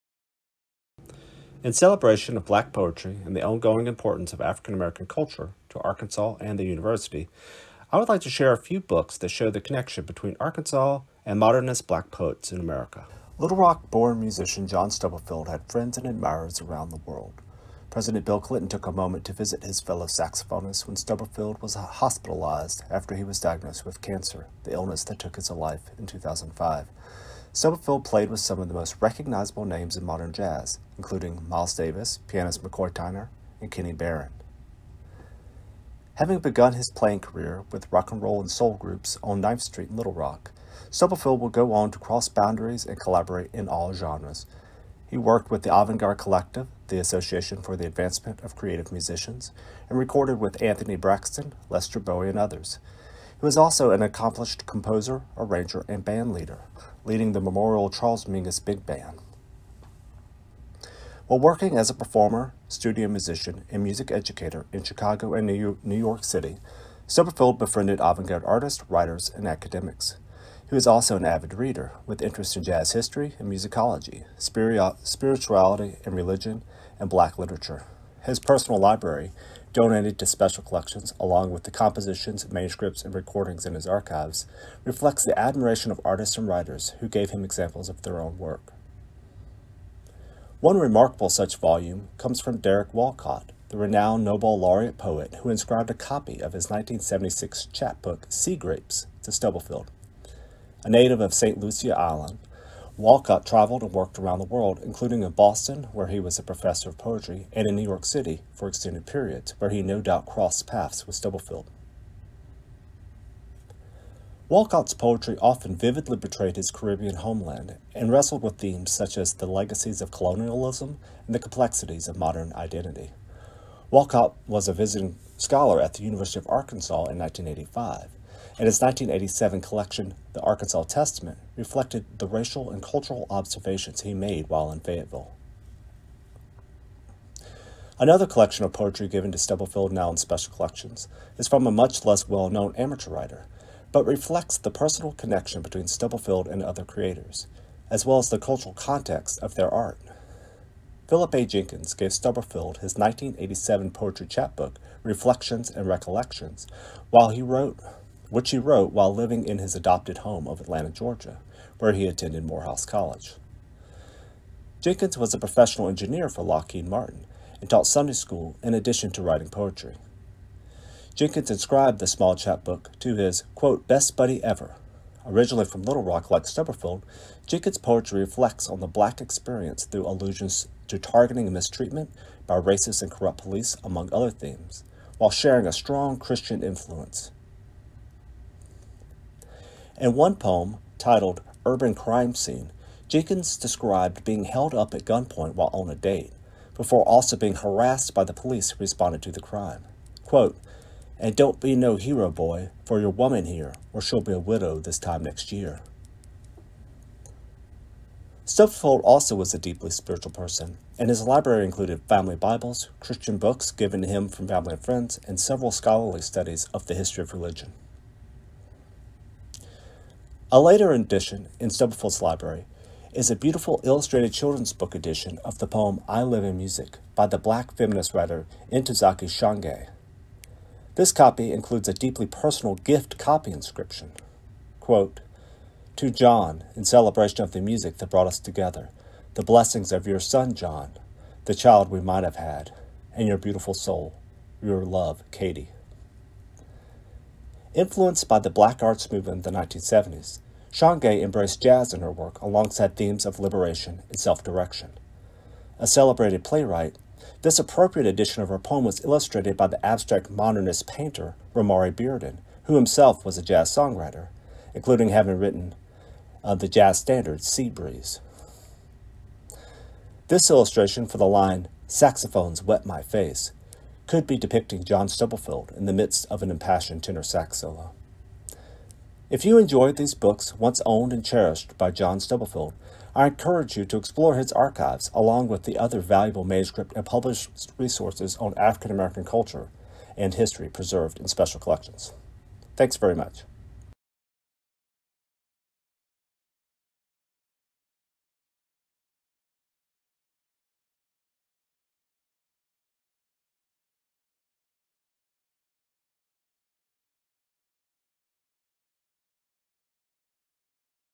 Jacob Stubblefield with the University of Arkansas Special Collections
Join archivists from Special Collections at the University of Arkansas in a video exploration of selections from their African American poetry collections. This program is part of Lift Every Voice: Why African American Poetry Matters, a national public humanities initiative of Library of America presented in partnership with The Schomburg Center for Research in Black Culture with generous support from The National Endowment for the Humanities, The Andrew W. Mellon Foundation, and Emerson Collective.